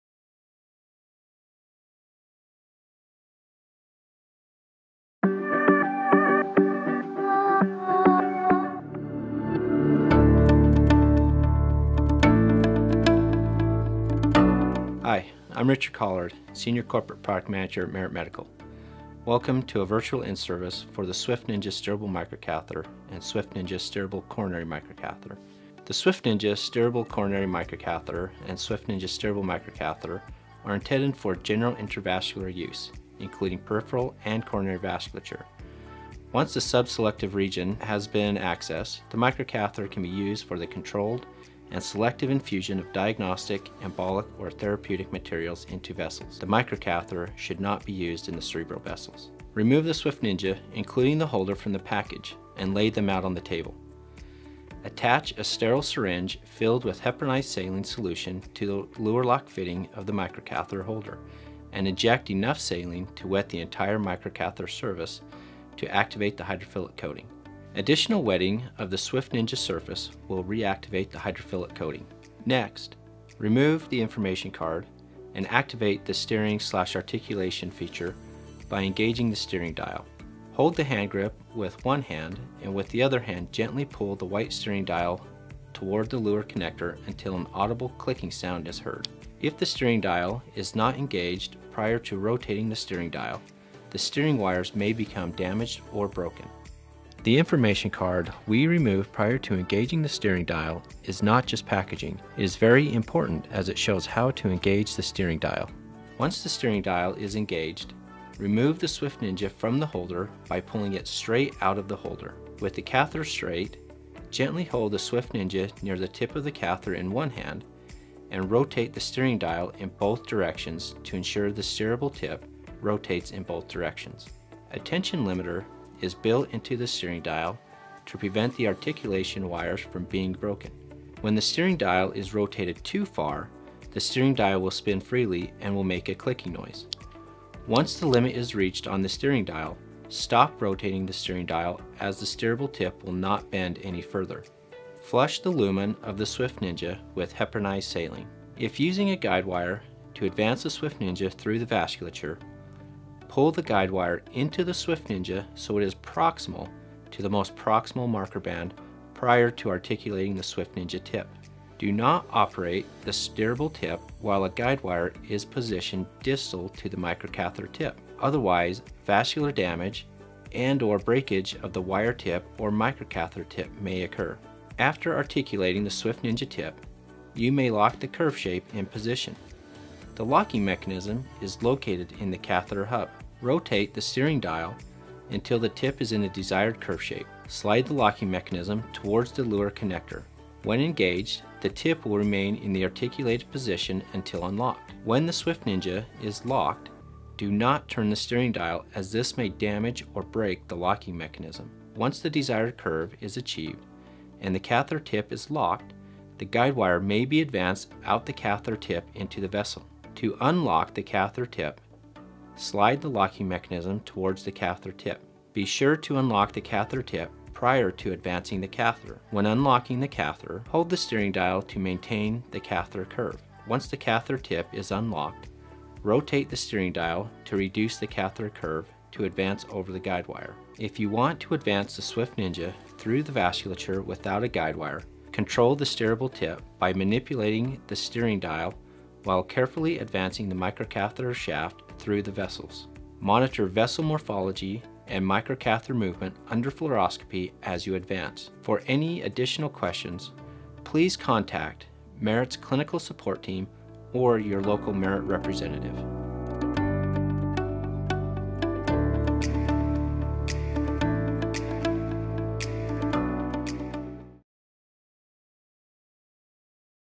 SwiftNINJA Steerable Microcatheter In-Service Video
When you need speed, delivery, and control in your patient procedures, reach for the SwiftNINJA Steerable Microcatheter. Discover how this product can help you achieve your clinical goals in this useful in-service video.